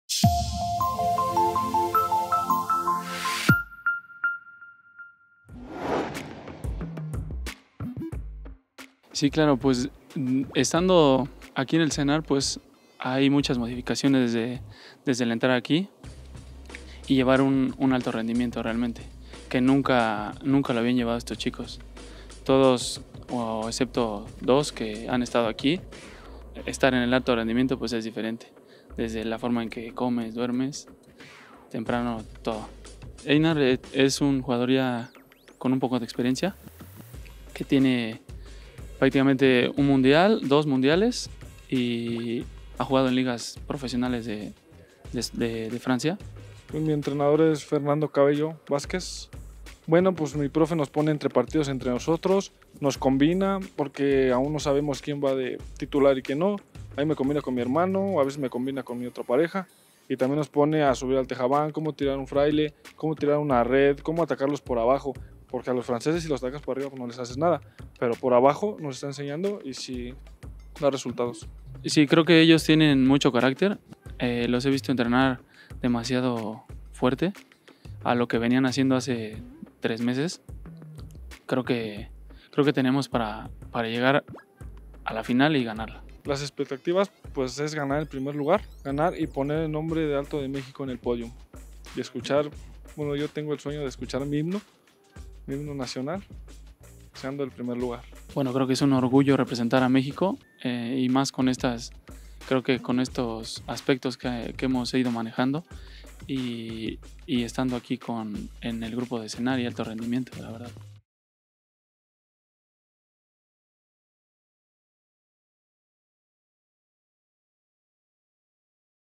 Entrenador Fernando Cabello y pelotari Einar Mundo Noxpanco - Selección Nacional de Trinquete
La selección nacional de trinquete modalidades mano individual y mano parejas varonil, está lista para participar en la Liga de Naciones 2024 que se realizará del 22 al 28 de septiembre en París, Francia.